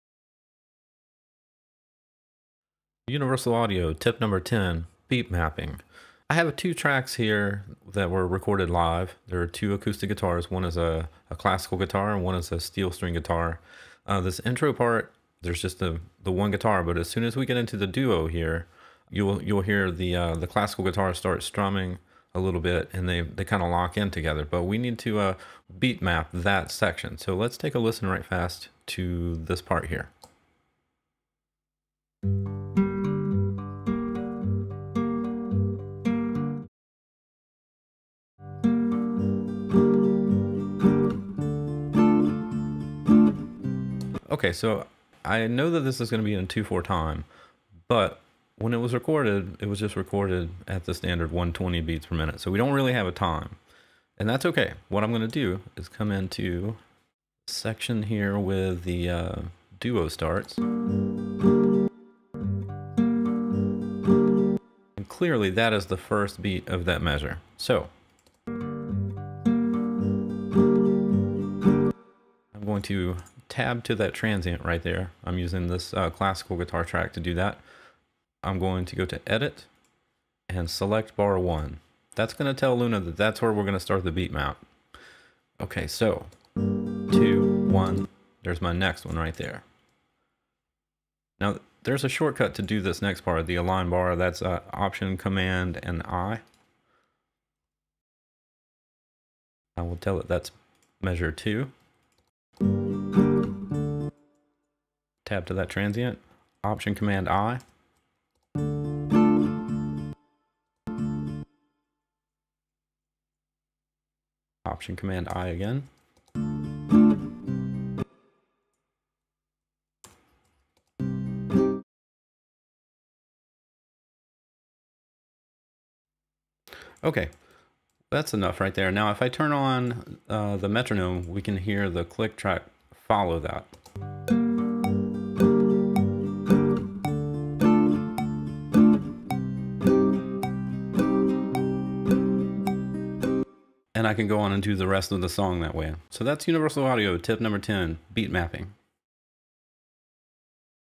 Universal Audio LUNA - Tip #10 - Beat Mapping
How to use Console alongside Universal Audio's Recording System LUNA.